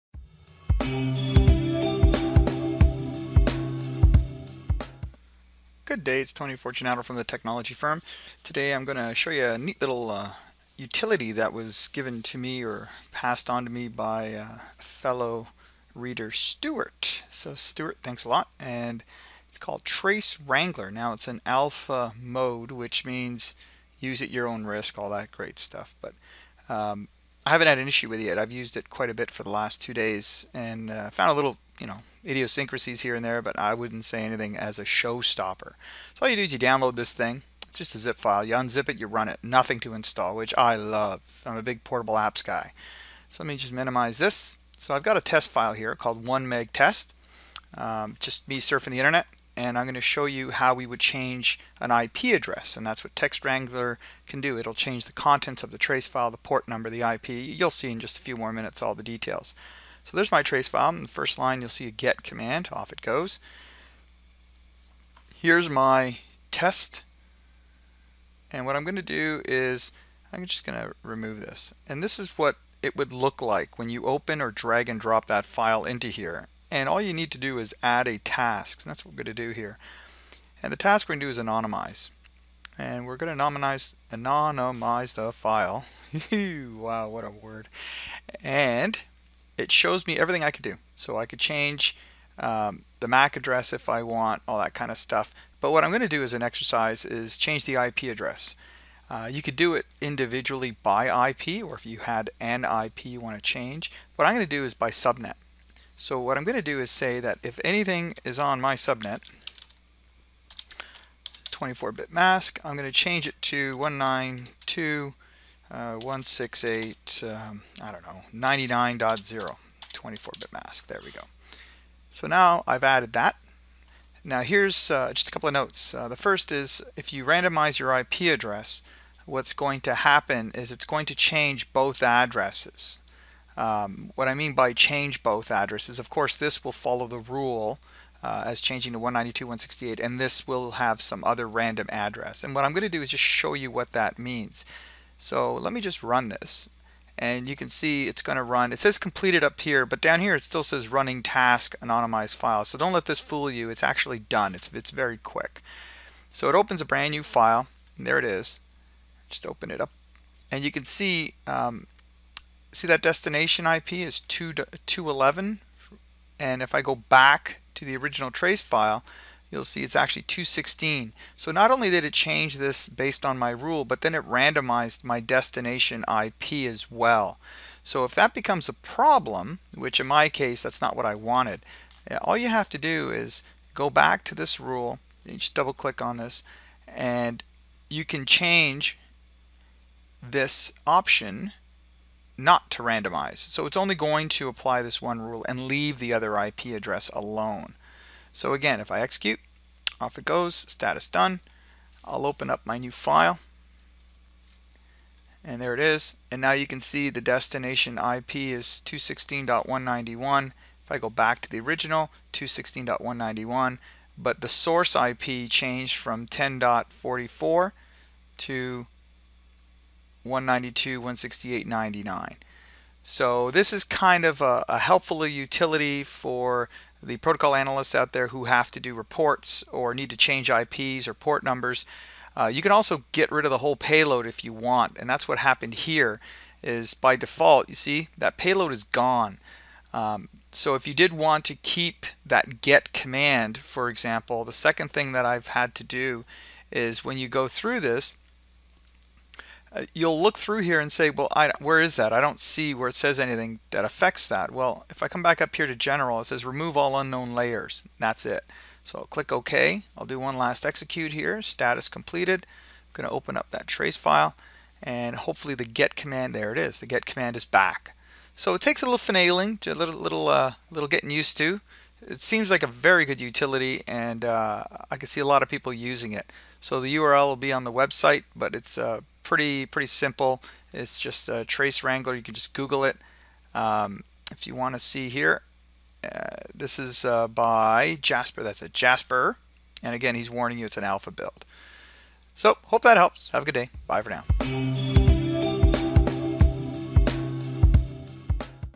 Tracewrangler Review - Edit your trace files
LIKE if you want more videos on this topic

Many of my clients or I can not share trace files because sensitive public ip addresses, port numbers or data is easliy seen

within the trace. Unfortunately many vendors would benefit from the trace to address a performance or bug within their

product.

In the end I typically resort to blurred screen captures, a lot of writing and more phone conversations to convey what I am

trying to explain.  Because of this extra effort, I must admit that as a general rule, I shy away from offering trace files.